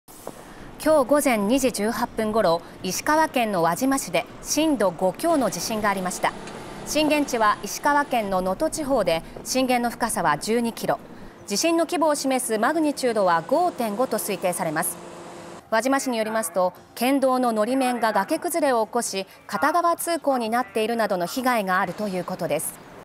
石川･輪島で震度５強
きょう午前２時18分ごろ、石川県の輪島市で震度５強の地震がありました。
震源地は石川県の能登地方で、震源の深さは12キロ、地震の規模を示すマグニチュードは5.5と推定されます。
輪島市によりますと、県道ののり面が崖崩れを起こし、片側通行になっているなどの被害があるということです。